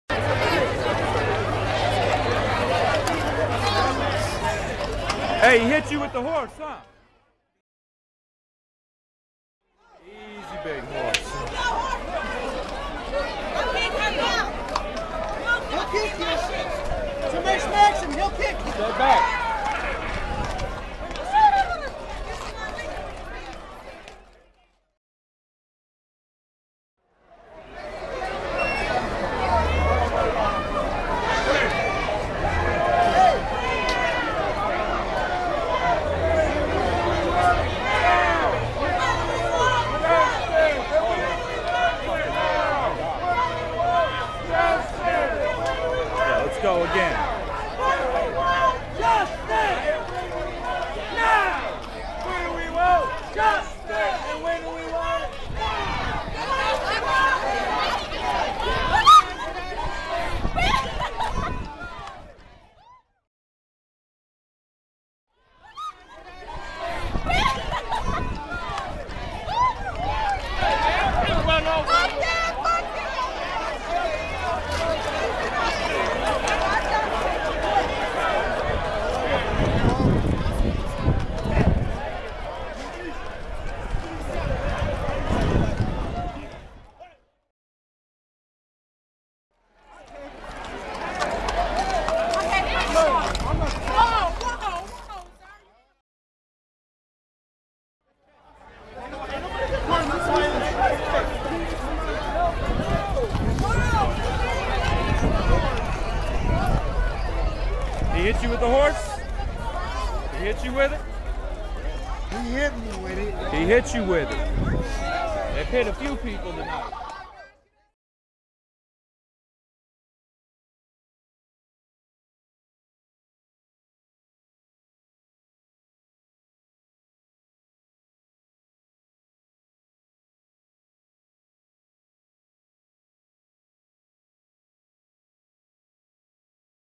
Wichita Police Horse Violence in Old Town
Every weekend, in Old Town, Wichita Police officers mount horses and ride into crowds of people leaving the city's bar district, hitting people left and right without regard. The officers target the c